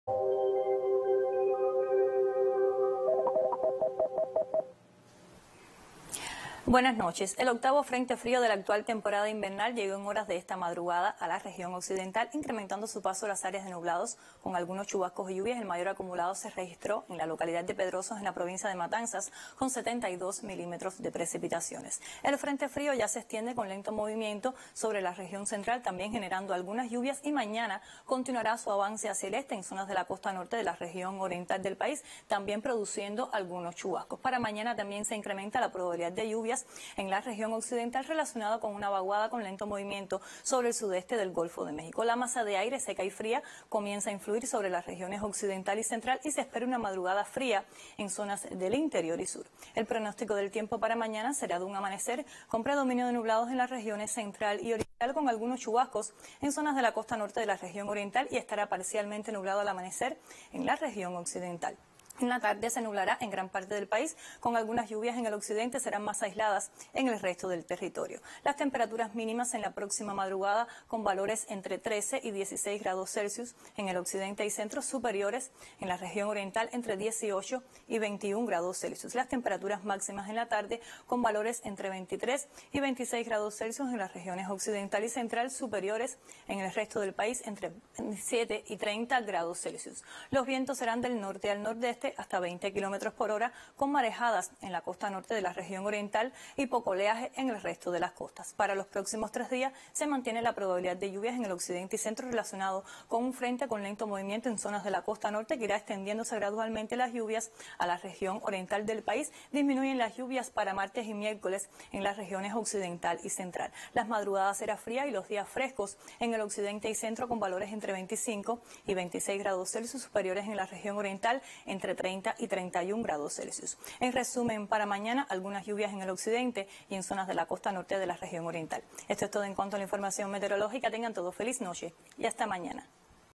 El Tiempo en Cuba: condiciones invernales en occidente y centro
Estará mayormente nublado en centro y oriente con algunos chubascos y lluvias en zonas de la costa norte oriental. En el occidente de Cuba estará parcialmente nublado y se nublará desde el final de la tarde con AlgunosChubascos. En el resto del territorio las lluvias serán aisladas.La madrugada será fría con temperaturas mínimas entre 13 y 16 grados Celsius en el interior de la mitad occidental, entre 19 y 22 grados Celsius en el resto del país.